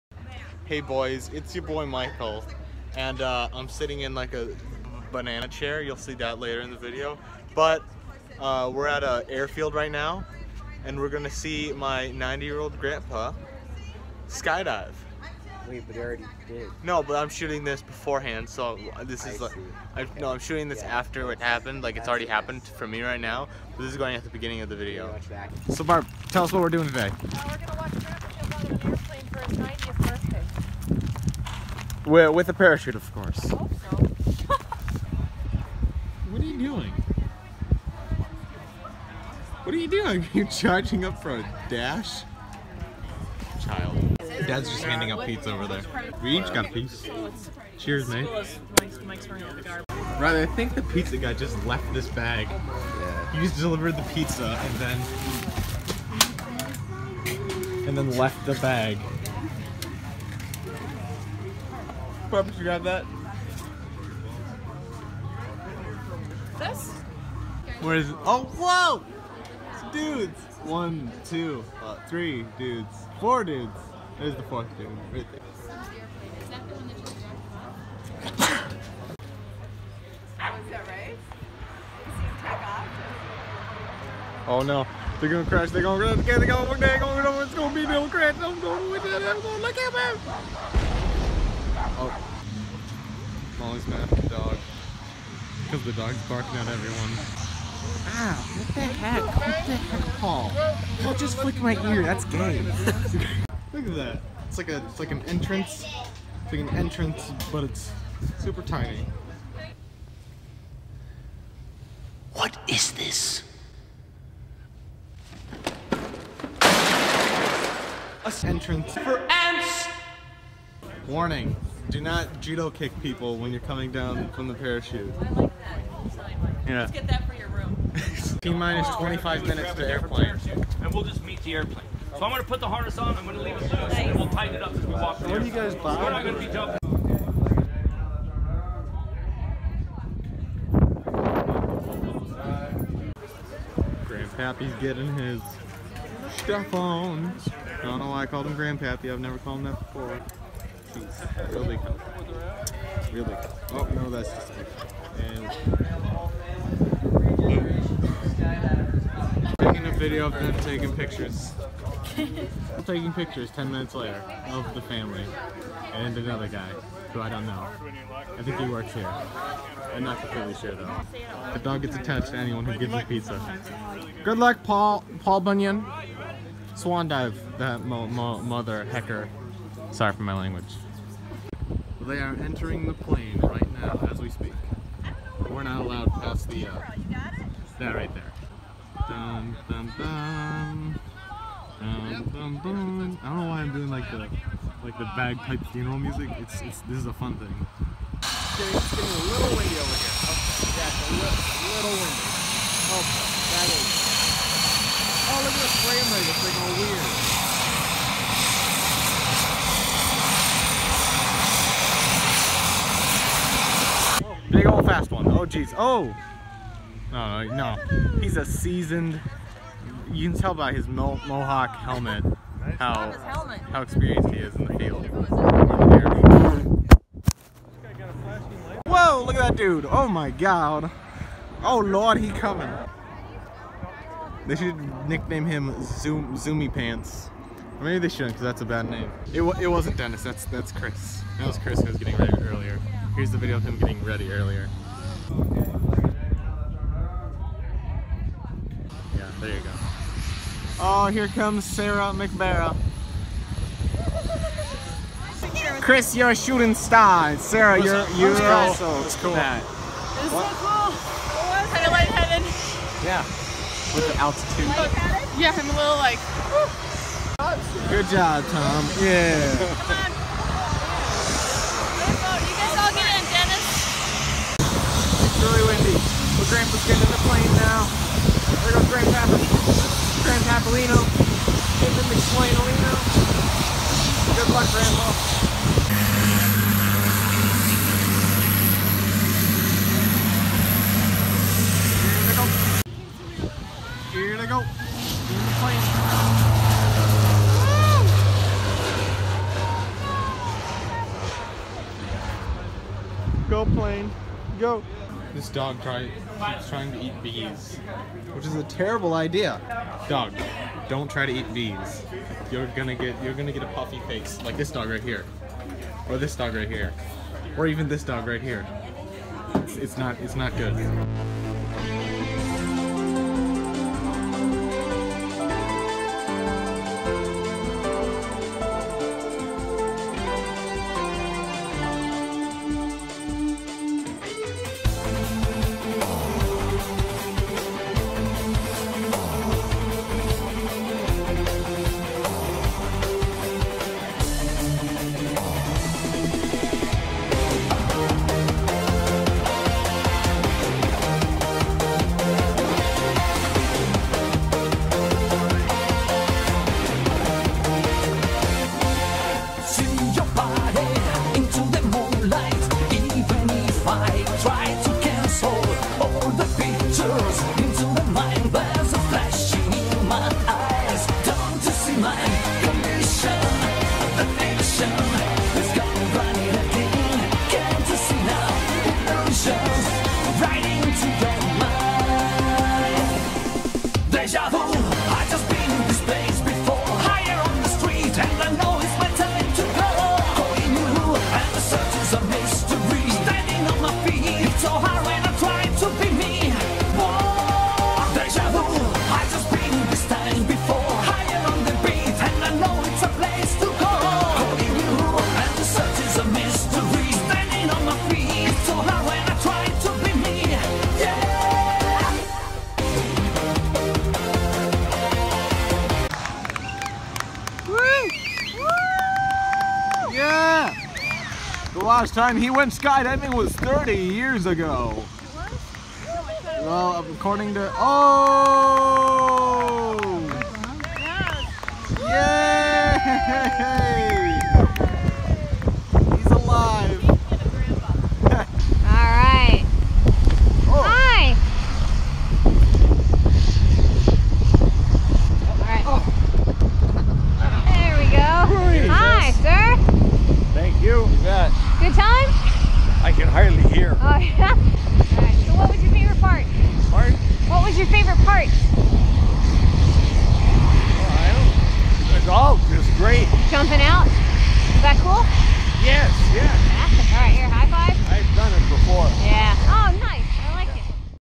My 90 Year Old Grandpa Goes Skydiving!
My grandpa decided to go skydiving for his 90th birthday.